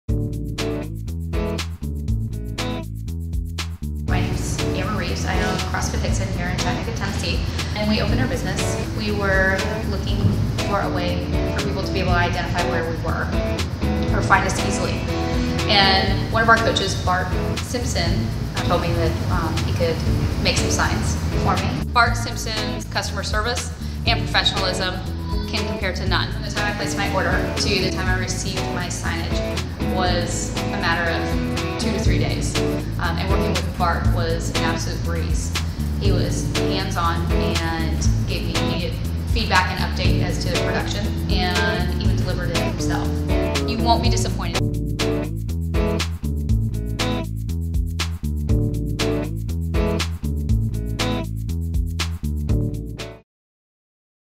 Flywheel Testimonial from CrossFit Hixson
Amber Reeves is the owner of CrossFit in Hixson, TN and a client of Flywheel. We helped her with her signage to promote her business in its surrounding area. Listen to her story to learn more about how Flywheel added momentum to Amber's marketing efforts.